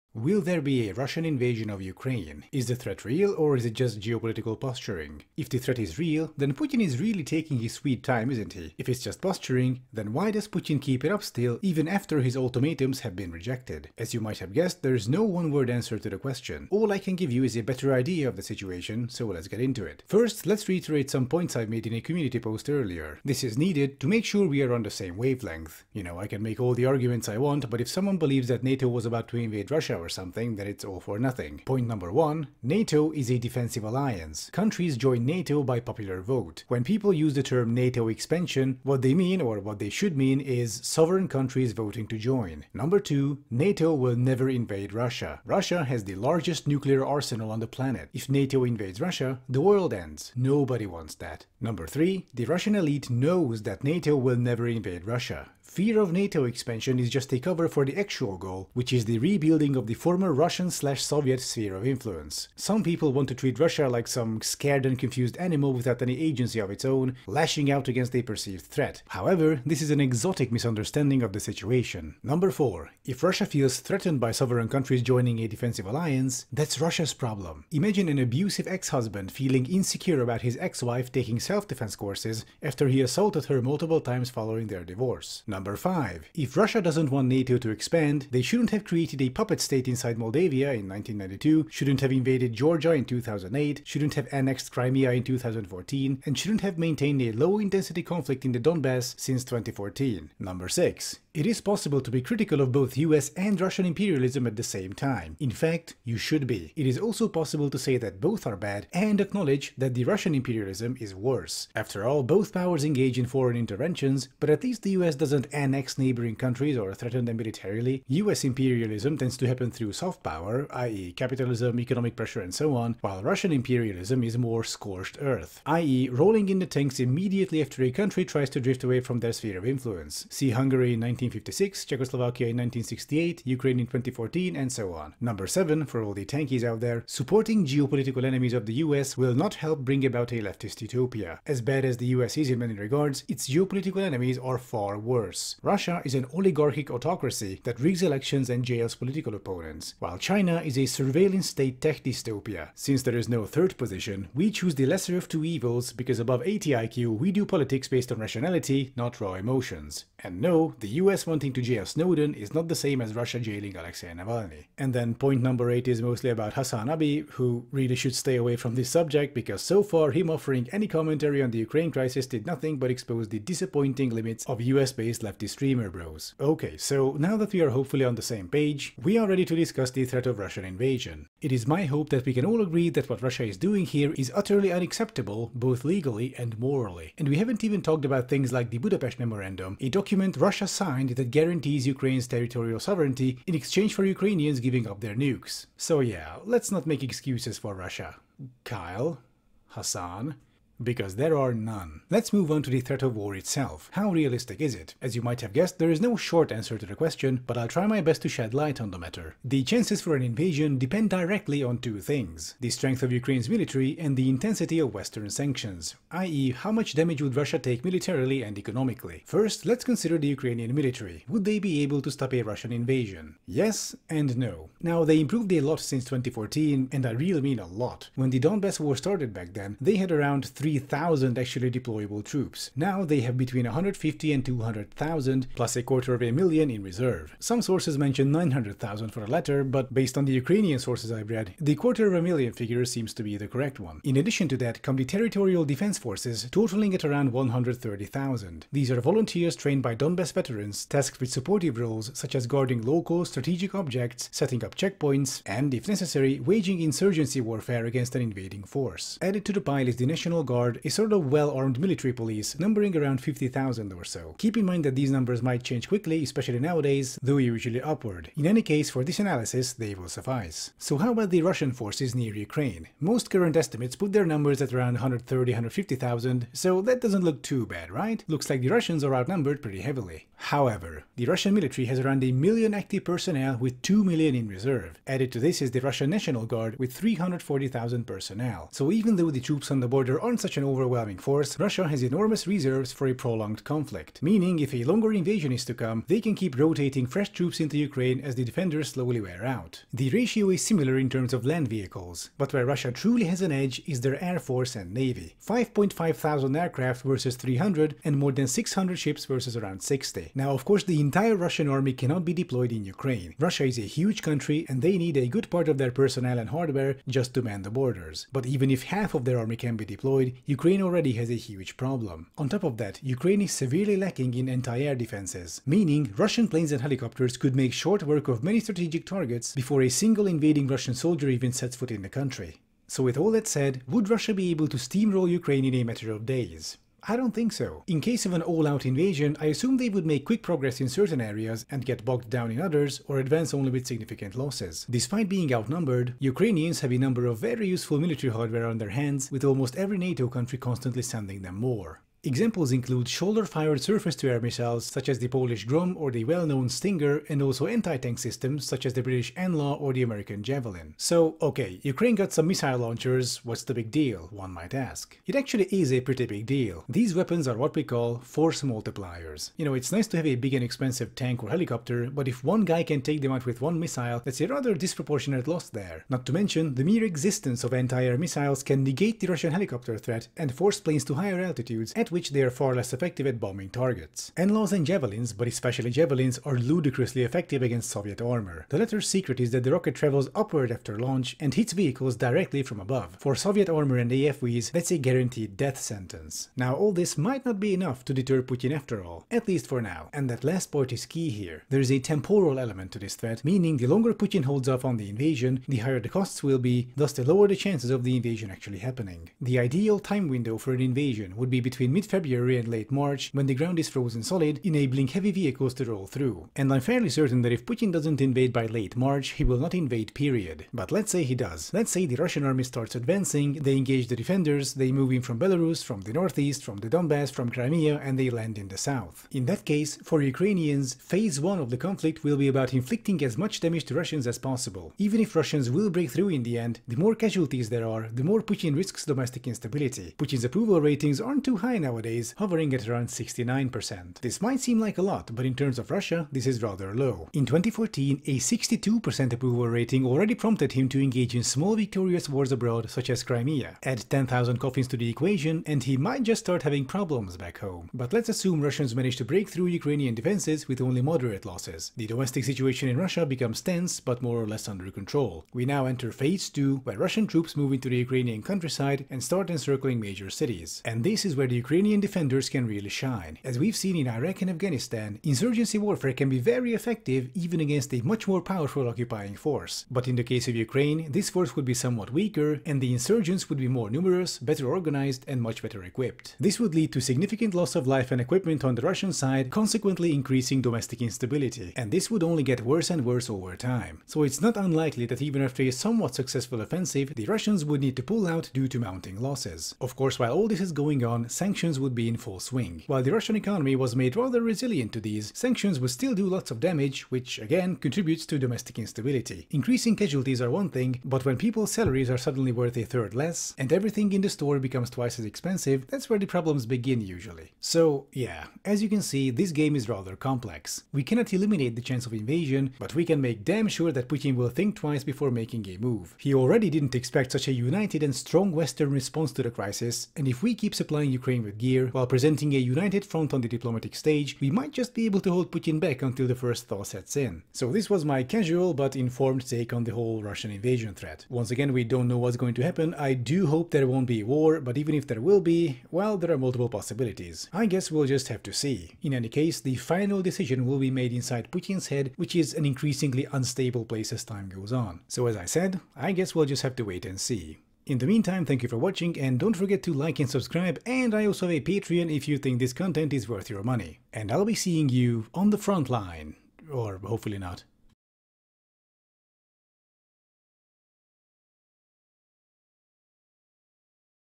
Assessing The Russian Invasion Threat
Hey, it's your favorite armchair general, back with a brief take on the Russian invasion threat!

For a far more detailed analysis I recommend you check out this article:

I won't be doing a longer, more detailed analysis for now, things are changing too rapidly for that.